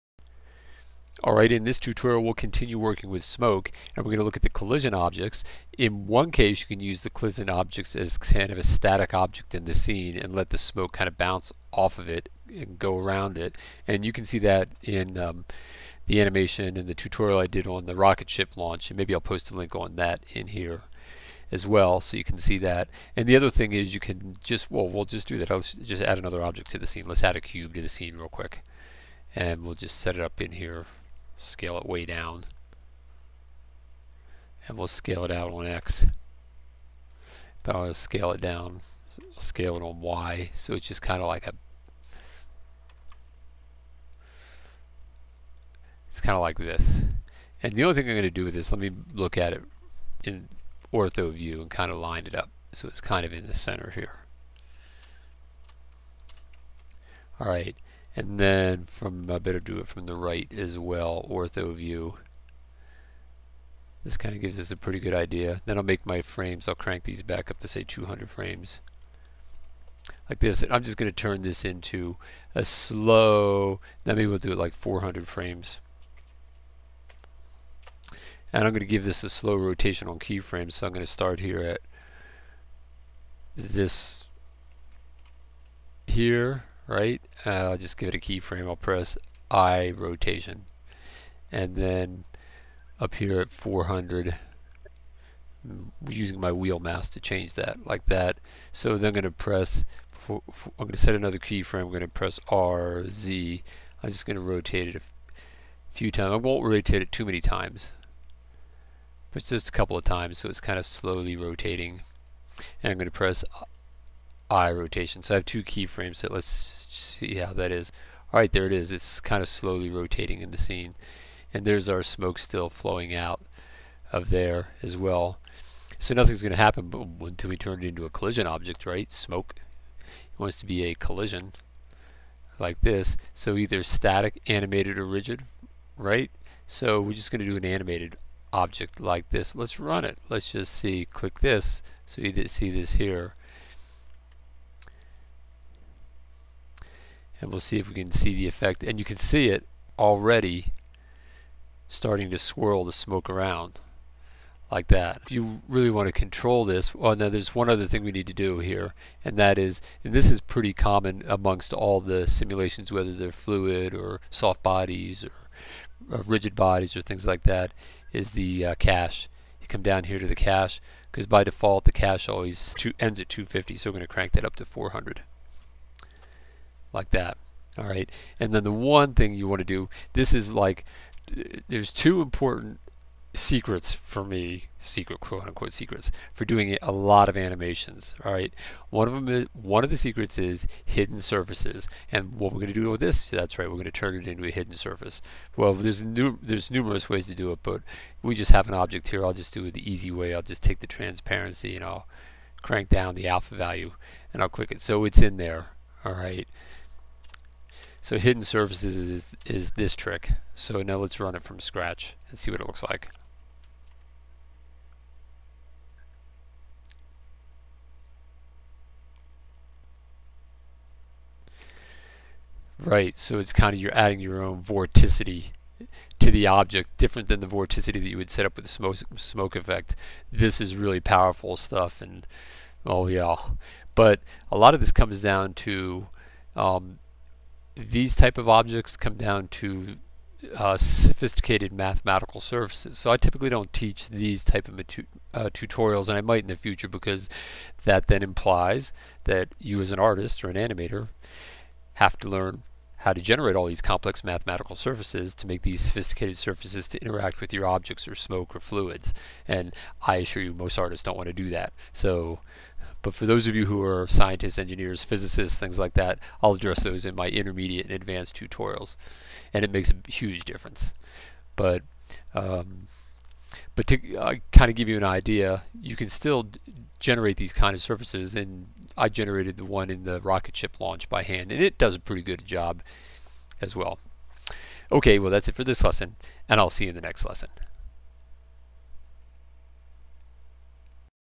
Blender Tutorial - Powerful Twisting Smoke with Collision Objects
In this Blender tutorial, I show you how easy it is to add powerful twisting, or other animated effects, to your smoke simulations.